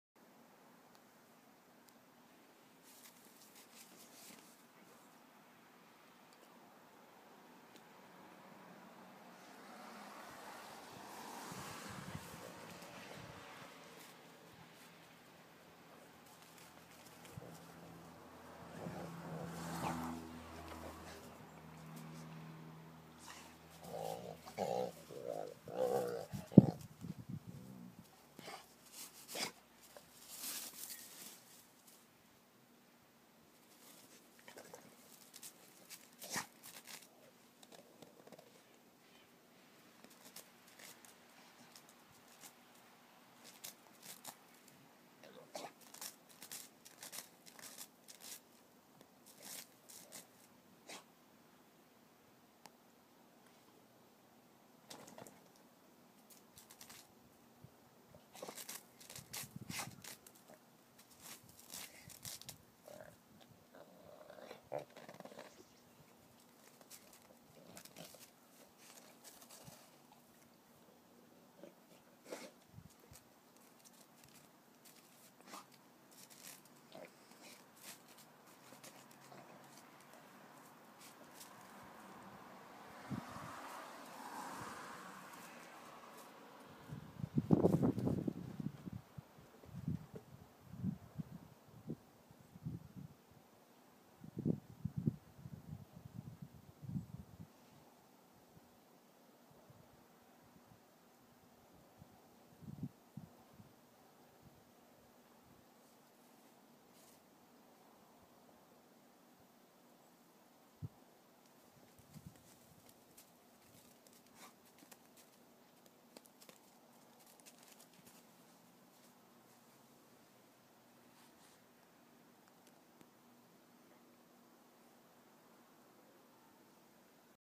silly kalden
kalden rolling in the snow